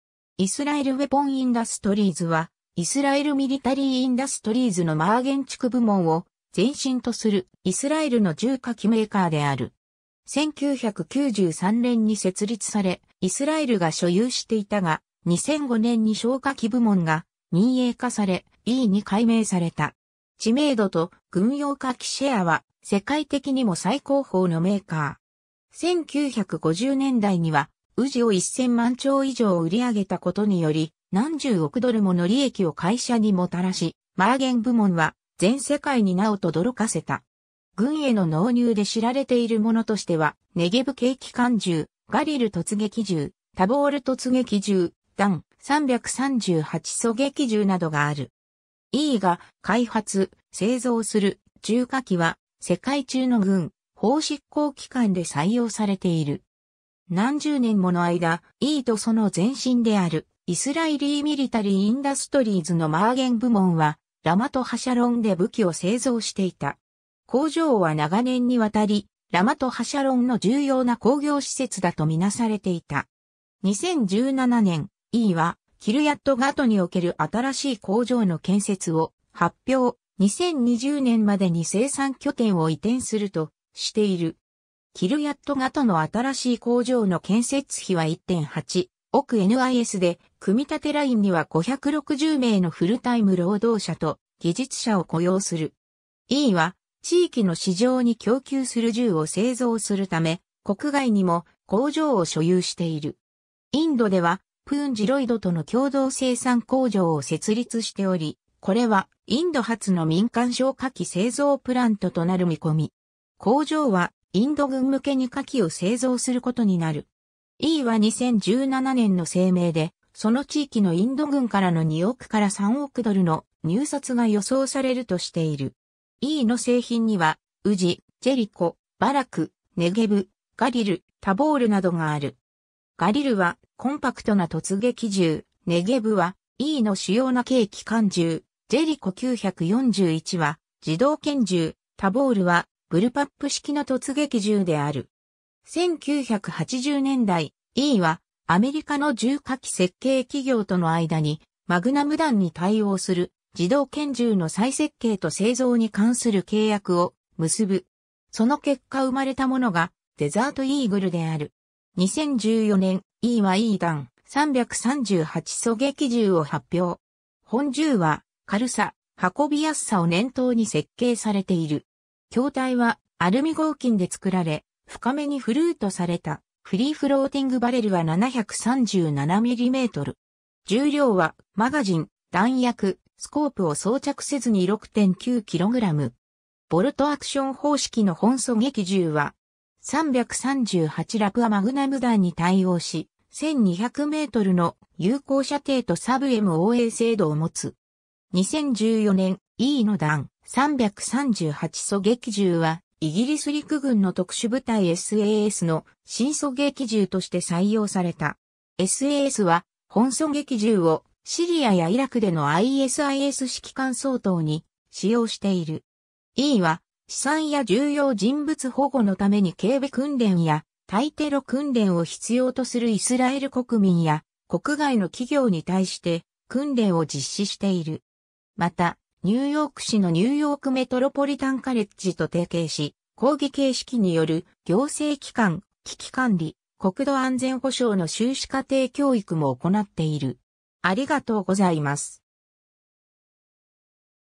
イスラエル・ウェポン・インダストリーズ
イスラエル・ミリタリー・インダストリーズ
イスラエル・ウェポン・インダストリーズ (Israel Weapon Industries, IWI)は、イスラエル・ミリタリー・インダストリーズのマーゲン地区部門を前身とする、イスラエルの銃火器メーカーである。
 1993年に設立され、イスラエルが所有していたが、2005年に小火器部門が民営化され、IWIに改名された。
 知名度と軍用火器シェアは、世界的にも最高峰のメーカー。
1950年代には、UZI を1000万丁以上を売り上げたことにより、何十億ドルもの利益を会社にもたらし、マーゲン部門は全世界に名を轟かせた。
軍への納入で知られているものとしては、ネゲヴ軽機関銃、ガリル突撃銃、タボール突撃銃、DAN .338狙撃銃などがある。
 IWIが開発、製造する銃火器は世界中の軍、法執行機関で採用されている。
 何十年もの間、IWIとその前身であるIsraeli Military Industriesのマーゲン部門は、ラマト・ハシャロンで武器を製造していた。
工場は長年に渡り、ラマト・ハシャロンの重要な工業施設だとみなされていた。
2017年、IWIはキルヤット・ガトにおける新しい工場の建設を発表、2020年までに生産拠点を移転するとしている。
キルヤット・ガトの新しい工場の建設費は1.8億NISで、組み立てラインには560名のフルタイム労働者と技術者を雇用する。
 IWIは、地域の市場に供給する銃を製造するため、国外にも工場を所有している。
インドでは、Punj Lloydとの共同生産工場を設立しており(プラントの49%をIWIが、51%をPunj Lloydが所有)、これはインド初の民間小火器製造プラントとなる見込み。
工場はインド軍向けに火器を製造することになる。
IWIは2017年の声明で、その地域のインド軍からの2億～3億ドルの入札が予想されるとしている。
 IWIの製品には、UZI、ジェリコ、バラク、ネゲヴ、ガリル、タボール などがある。
ガリルはコンパクトな突撃銃、ネゲヴはIWIの主要な軽機関銃、ジェリコ941は自動拳銃、タボールはブルパップ式の突撃銃である。
 1980年代、IWIはアメリカの銃火器設計企業との間に、マグナム弾(.44 マグナム、 .357 マグナム、.50 AE)に対応する自動拳銃の再設計と製造に関する契約を結ぶ。
その結果生まれたものが、デザートイーグルである。
 2014年、IWIはIWI DAN .338狙撃銃を発表。
 本銃は軽さ、運びやすさを念頭に設計されている。
筐体はアルミ合金で作られ、深めにフルートされたフリーフローティングバレルは737mm。
重量はマガジン、弾薬、スコープを装着せずに6.9 Kg 。
ボルトアクション方式の本狙撃銃は.338ラプア・マグナム弾に対応し、1200mの有効射程とサブMOA精度を持つ。
 2014年、IWIのDAN .338 狙撃銃はイギリス陸軍の特殊部隊SAS の新狙撃銃として採用された。
SASは、本狙撃銃をシリアやイラクでのISIS指揮官掃討に使用している。
 IWIは、資産や重要人物保護のために警備訓練や対テロ訓練を必要とするイスラエル国民や国外の企業に対して、訓練を実施している。
また、ニューヨーク市のニューヨークメトロポリタンカレッジ(MCNY)と提携し、講義形式による行政機関、危機管理、国土安全保障の修士課程教育も行っている。